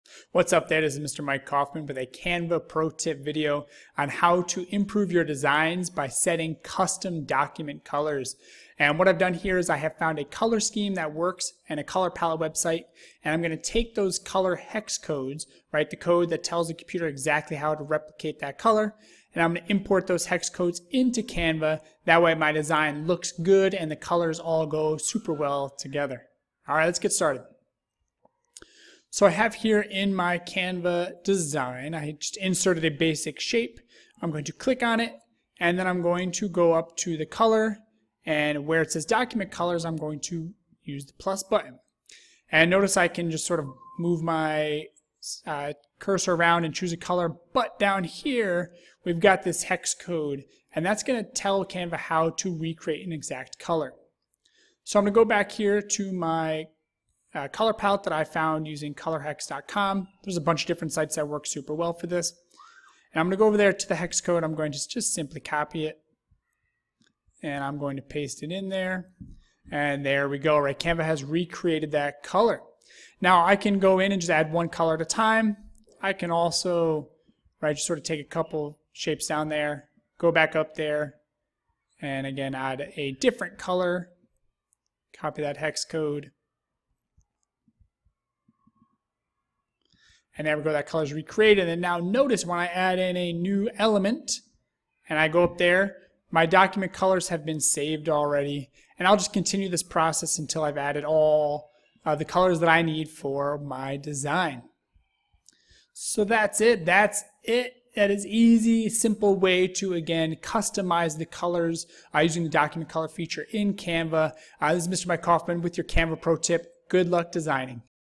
Setting Custom Document Colors in Canva
How to use color pallet hex codes to set custom documents colors in Canva to help personalize your designs!